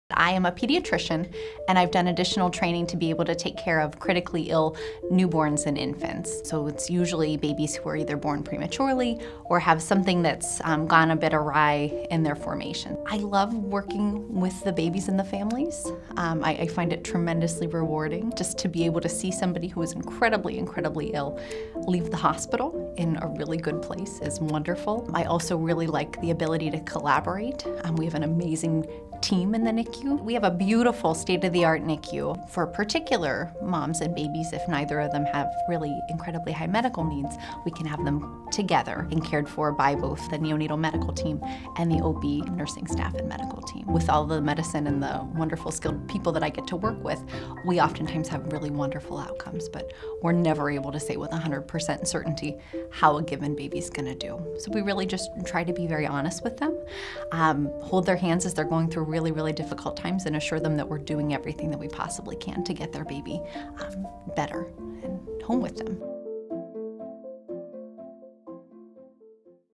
Meet Yale Medicine Neonatologist Lindsay Johnston, MD, MEd
Lindsay Johnston, MD, is a neonatologist who treats critically ill infants in the Newborn Intensive Care Unit (NICU) at Yale New Haven Children’s Hospital. Her patients, many of whom are born prematurely or have congenital anomalies, are incredibly fragile. “It’s extremely rewarding to be able to help these babies when they are so ill, and send them home doing very, very well,” says Dr. Johnston, who knew she wanted to be a doctor since she was 4 years old. “I thought I would be a general pediatrician, but when I went through residency training, I was just drawn to the NICU, the practice of medicine that goes on there, and the impact it has.” Dr. Johnston says she also enjoys guiding families through difficult times. “We can never make promises to families about how things are going to go, but we are honest and hold their hands as they’re going through really difficult times,” she says. “I assure them that we’re doing everything we possibly can to get their baby better and home with them.” As an associate professor of pediatrics (neonatology) and director of Yale Medicine’s training program in neonatal-perinatal medicine, Dr. Johnston is also involved in the education of trainees and medical staff, often through simulation-based sessions. She is particularly interested in training related to Extracorporeal Membrane Oxygenation (ECMO), a specialized machine that acts as an artificial heart and lung for a prolonged period of time.